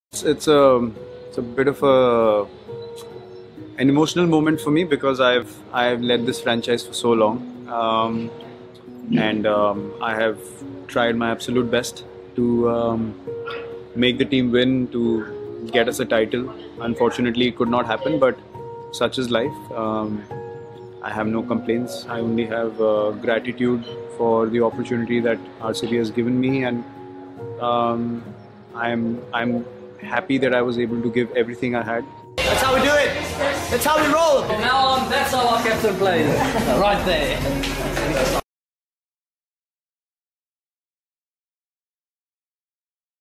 Virat Kohli Emotional Moment After RR vs RCB Match | Virat Kohli Very Disappoint Us by Himself
Virat Kohli Emotional Moment After RR vs RCB Match | Virat Kohli Very Disappoint Us by Himself | Sad Moment Of Virat Kohli | Kohli Will Never Let Down Come With Next Year And Win The Trophy Good Luck.